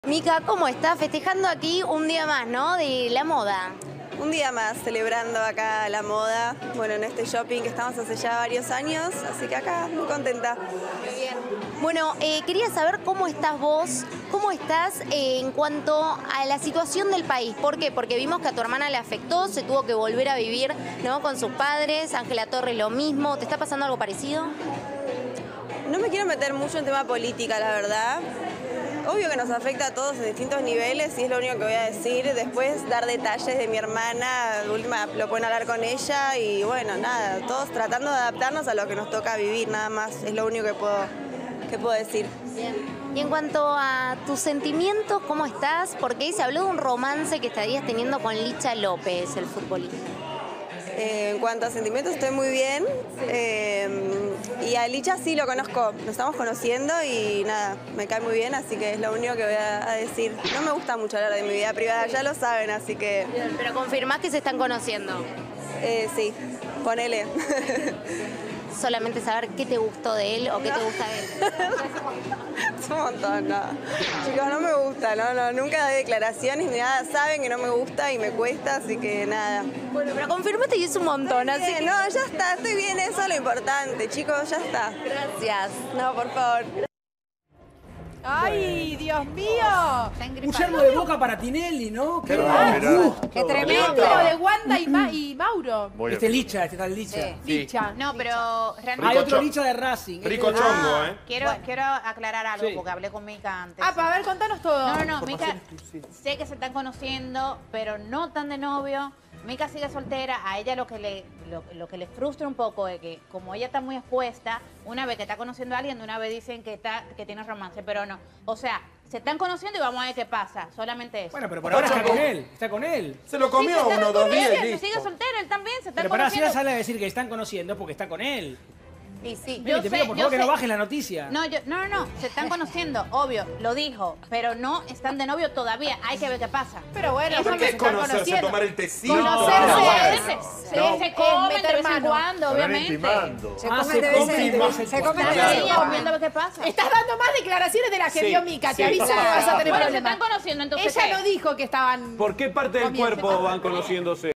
Mica Tinelli, botinera
Conversamos con Mica Tinelli sobre su nueva relación con Licha López